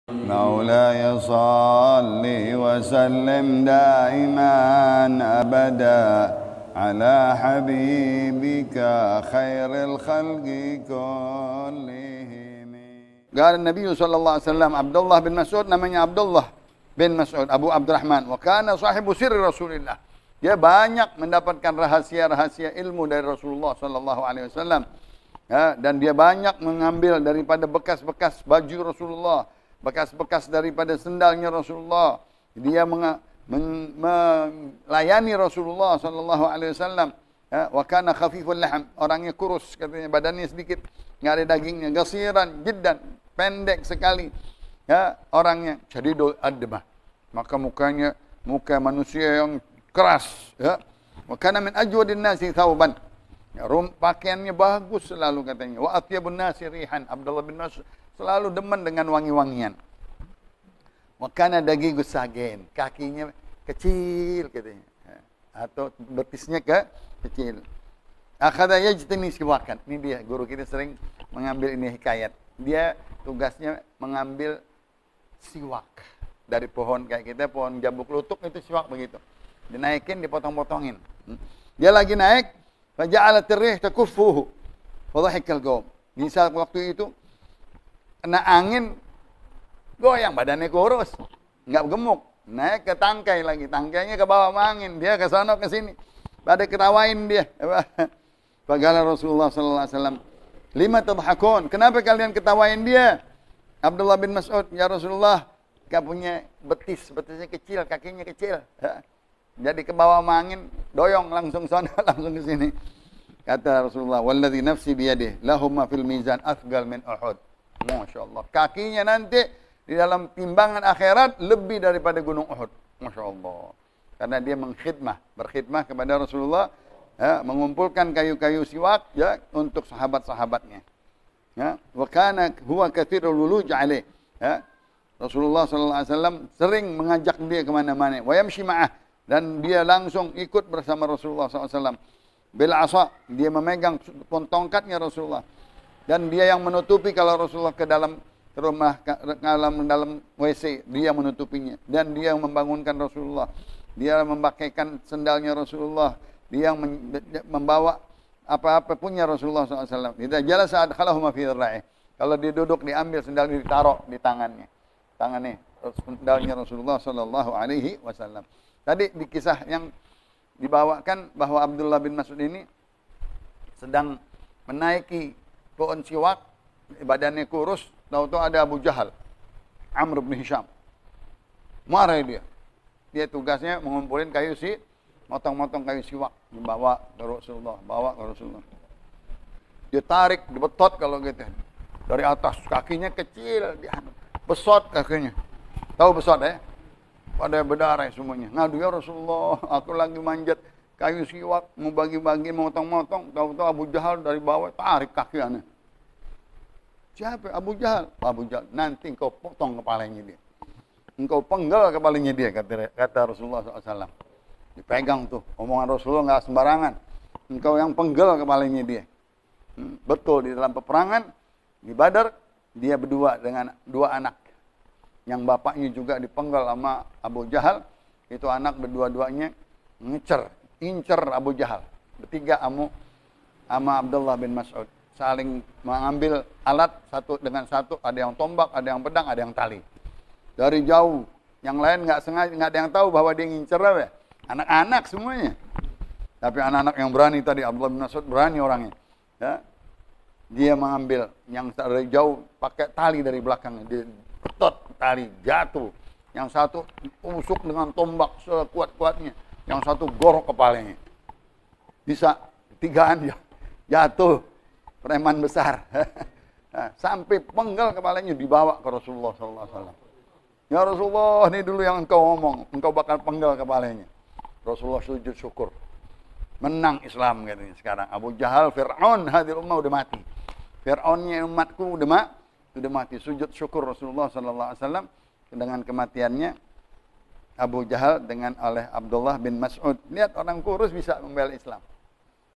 KISAH ABDULLAH BIN MAS'UD DITARIK JATUH DARI POHON OLEH ABU JAHAL,DAN PENGGAL KEPALA ABU JAHAL.
HABIB HUD ALATAS || PEMBAHASAN KITAB TANQIHUL QAULIL HASIST.

BABEHERDOGAN || BILA INGIN MENGCOPY REKAMAN VIDEO INI HARAP IZIN TERLEBIH DAHULU SEMOGA KITA SELALU DALAM KEBERKAHAN AAMIIN.